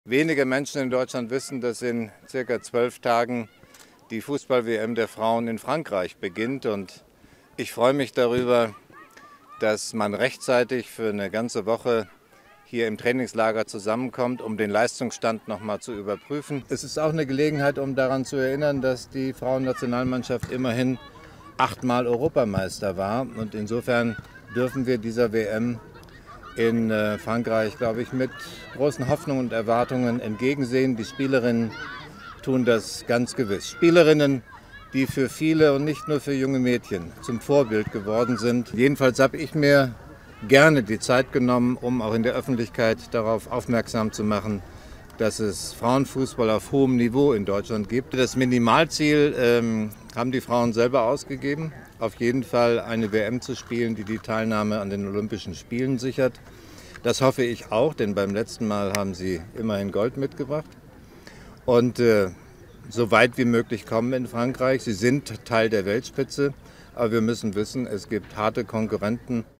Steinmeier vor Frauen-WM: "Spielerinnen sind Vorbilder geworden"
Bundespräsident Frank-Walter Steinmeier blickt mit viel Vorfreude auf die kommende Frauenfußball-WM in Frankreich. Das deutsche Staatsoberhaupt hofft auf ein erfolgreiches Auftreten des Teams von Bundestrainerin Martina Voss-Tecklenburg.